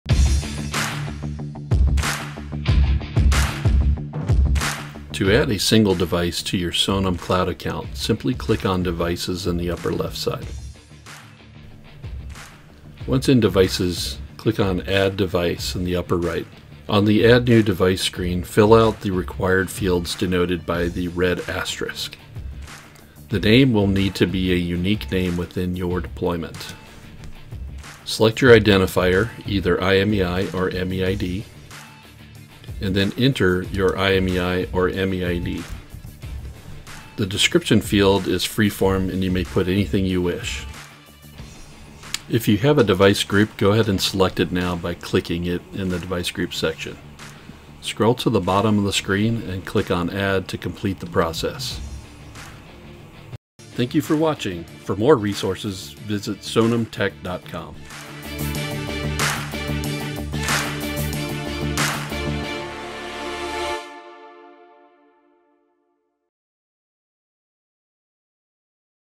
Sonim Cloud - Devices - Add Device
How to add a single device on Sonim Cloud.